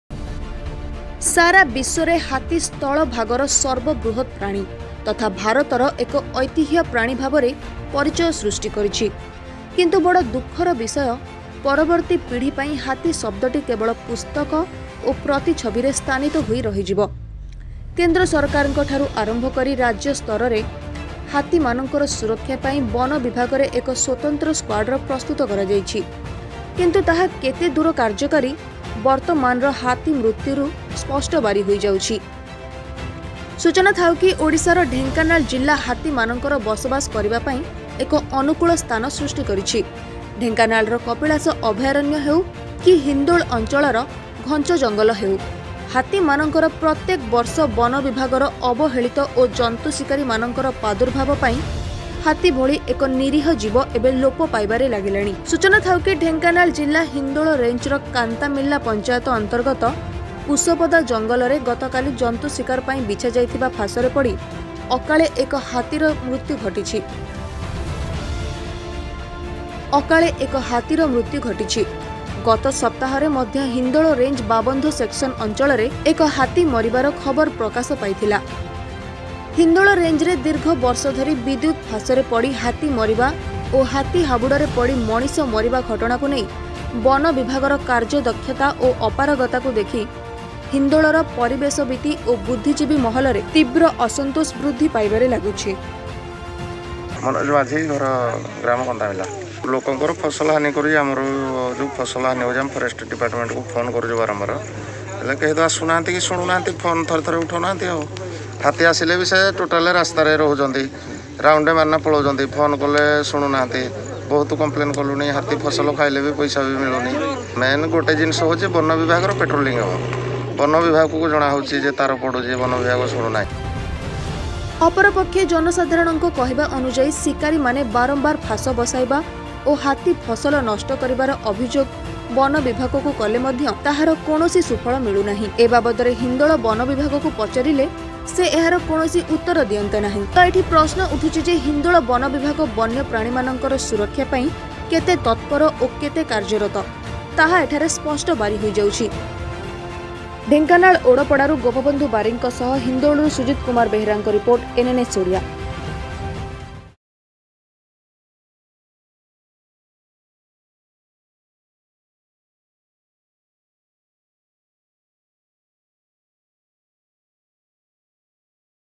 ଚକ୍ରାନ୍ତ କରି ହାତୀ ହତ୍ୟା କରୁଛନ୍ତି ଶିକାରୀ
ସାରା ବିଶ୍ୱରେ ହାତୀ ସ୍ଥଳ ଭାଗର ସର୍ବ ବୃହତ ପ୍ରାଣୀ ଭାବେ ପରିଚୟ ସୃଷ୍ଟି କରିଛି। କିନ୍ତୁ ବଡ଼  ଦୁଃଖ ର ବିଷୟ ପରବର୍ତ୍ତୀ ପିଢ଼ି ପାଇଁ ହାତୀ ଶବ୍ଦ ଟି କେବଳ ପୁସ୍ତକ ଓ ପ୍ରତିଛବି ରେ ସ୍ଥାନିତ ହୋଇ ରହିଯିବ । କେନ୍ଦ୍ରସରକାର ଙ୍କ ଠାରୁ ଆରମ୍ଭ କରି ରାଜ୍ୟ ସ୍ଥର ରେ ମଧ୍ୟ ହାତୀ ମାନଙ୍କର ସୁରକ୍ଷା ପାଇଁ ବନ ବିଭାଗ ରେ ଏକ ସ୍ୱତନ୍ତ୍ର ସ୍କ୍ୱାର୍ଡ ର ପ୍ରସ୍ତୁତ କରାଯାଇଛି କିନ୍ତୁ ତାହା କେତେ ଦୂର କାର୍ଯ୍ୟକାରୀ ବର୍ତ୍ତମାନ ର ହାତୀ ମୃତ୍ୟ ରୁ ସ୍ପଷ୍ଟ ବାରି ହେଇଯାଉଛି।  ସୂଚନା ଥାଉ କି ଓଡିଶା ର ଢେଙ୍କାନାଳ ଜିଲା ହାତୀମାନଙ୍କ ର ବସବାସ କରିବା ପାଇଁ ଏକ ଅନୁକୂଳ ସ୍ଥାନ ସୃଷ୍ଟ କରିଛି । ଢେଙ୍କାନାଳ ର କପିଳାସ ଅଭୟାରଣ୍ୟ ହେଉ କି ହିନ୍ଦୋଳ ଅଞ୍ଚଳର ଘଞ୍ଚ ଜଙ୍ଗଲ ହେଉ  ହାତୀ ମାନଙ୍କର ପ୍ରତେକ ବର୍ଷ ବନ ବିଭାଗ ର ଅବହେଳିତ ଓ ଜନ୍ତୁ ଶିକାରୀ ମାନଙ୍କ ର ପ୍ରାଦୁର୍ଭାବ ପାଇଁ ହାତୀ ଭଳି ଏକ ନିରୀହ ଯିବ ଏବେ ଲୋପ ପାଇବାର ଲାଗିଲାଣି।
ସୂଚନା ଥାଉ କି ଢେଙ୍କାନାଳ ଜିଲା ହିନ୍ଦୋଳ   ରେଞ୍ଜର କଂନ୍ତାମିଲା ପଞ୍ଚାୟତ  ଅନ୍ତର୍ଗତ କୁସପଦା  ଜଙ୍ଗଲରେ ଗତ କାଲି  ଜନ୍ତୁ ଶିକାର ପାଇଁ ବିଛା ଯାଇଥିବା ଫାଶ ରେ ପଡି ଅକାଳେ ଏକ ହାତୀ ର  ମୃତ୍ୟ  ଘଟିଛି  ଗତ  ସପ୍ତାହ ମଧ୍ୟ  ହିନ୍ଦୋଳ  ରେଞ୍ଜ  ବାବନ୍ଧ  ସେକ୍ସନ  ଅଞ୍ଚଳରେ  ଏକ  ହାତୀ ମରିବାର ଖବର ପ୍ରକାଶ  ପାଇଥିଲା  l
ହିନ୍ଦୋଳ  ରେଞ୍ଜ ରେ ଦୀର୍ଘ ବର୍ଷ ଧରି    ବିଦୁତ ଫାଶ ରେ  ପଡି  ହାତୀ ମରିବା ଓ   ହାତୀ ହାବୁଡ଼ରେ ପଡି ମଣିଷ ମରିବା ଘଟଣା କୁ ନେଇ    ବନ ବିଭାଗର କାର୍ଯ୍ୟଦକ୍ଷତା  ଓ ଅପାରଗତ କୁ ଦେଖି  ହିନ୍ଦୋଳ ପରିବେଶବିତ ଓ  ବୁଦ୍ଧିଜୀବୀ ମହଲରେ  ତୀବ୍ର ଅସନ୍ତୋଷ ବୃଦ୍ଧି ପାଇବାରେ  ଲାଗୁଛି l
ଅପର ପକ୍ଷେ  ଜନସାଧାରଣ ଙ୍କ କହିବା ଅନୁସାରେ ଶିକାରୀ  ମାନେ  ବାରମ୍ବାର ଫାସ ବସାଇବା  ଓ ହାତୀ  ଫସଲ ନଷ୍ଟ କରିବା ର  ଅଭିଯୋଗ  ବନବିଭାଗ କୁ  କଲେ ମଧ୍ୟ  ତାହାର କୌଣସି  ଫଳପ୍ରଦ ହୁଏ ନାହିଁ। ଏ  ବାବଦରେ ହିନ୍ଦୋଳ  ବନବିଭାଗ  ଙ୍କୁ  ପଚାରିଲେ ସେ ଏହାର କୌଣସି ଉତ୍ତର ଦିଅନ୍ତିନି l ତ  ଏଠି ପ୍ରଶ୍ନ  ଉଠୁଛି ଯେ ହିନ୍ଦୋଳ ବନବିଭାଗ ବନ୍ୟ ପ୍ରାଣୀମାନଙ୍କ  ସୁରକ୍ଷା ପାଇଁ  କେତେ ତତ୍ପର ଓ  କେତେ କାର୍ଯ୍ୟରତ  ତାହା  ସ୍ପଷ୍ଟ  ବାରି ହେଇଯାଉଛି l